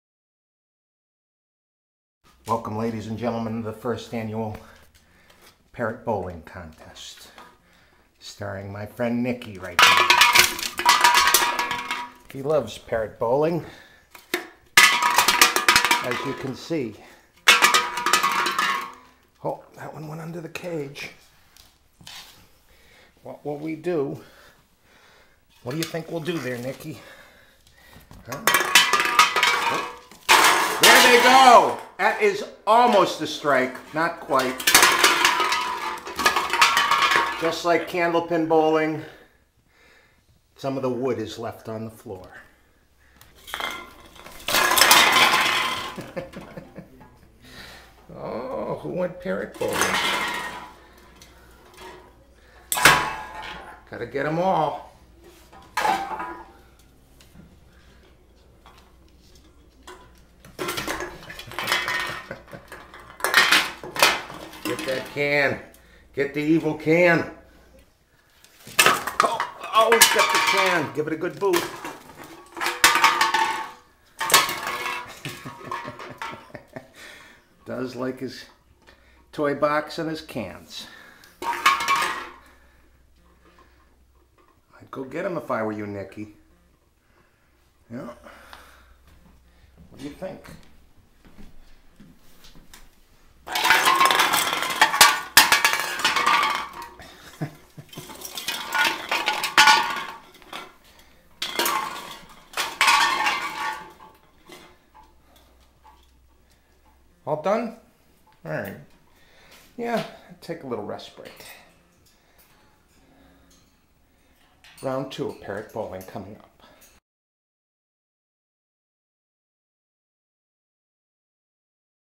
Parrot Bowling with Nicky
My Yellow Nape buddy Nicky likes to toss a can or 7 around out of his toy box.  The result is a lot like bowling.
Check more funny stuff out on my website, comedygolf.com